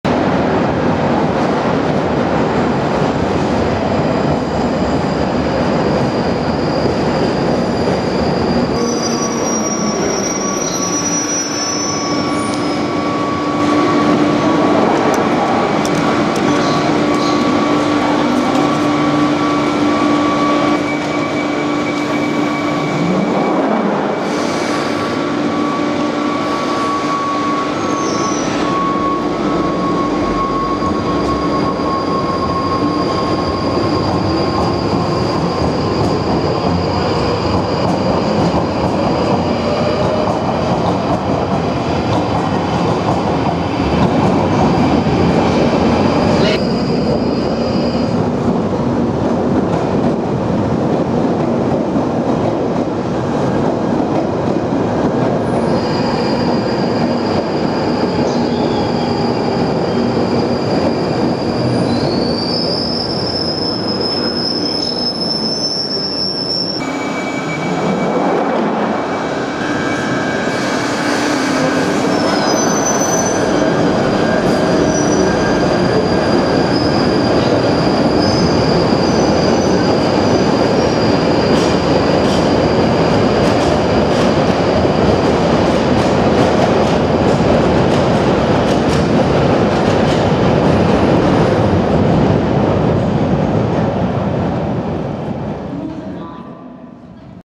Narrow Platforms Of London Underground: Clapham North Station Northern Line
The narrow Northern Line Platform at Clapham North Tube.